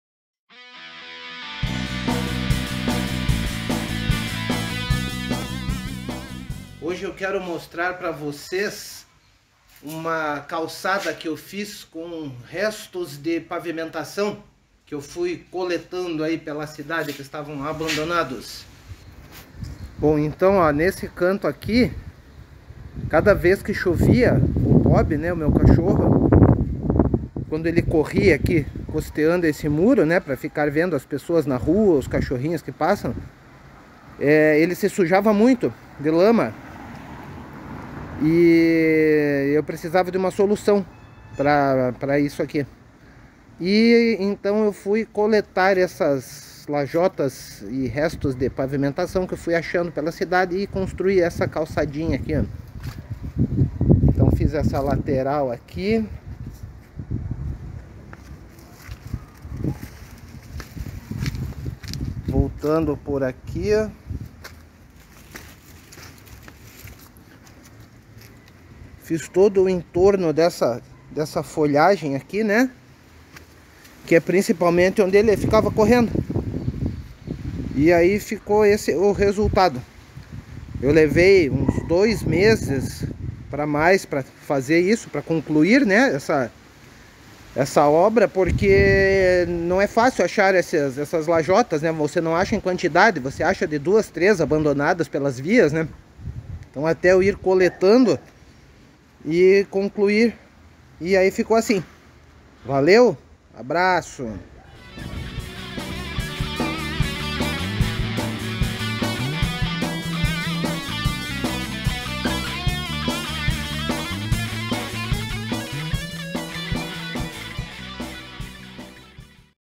Fiz uma pracinha com restos de obras.
Da pra fazer muitas coisas com objetos descartados e considerados "lixos". Fiz uma pracinha com restos de obra que coletei pela cidade, abandonados nas vias públicas, semienterrados em terrenos baldios ou descartados em caçambas. Esse foi o resultado! Abraço a todos!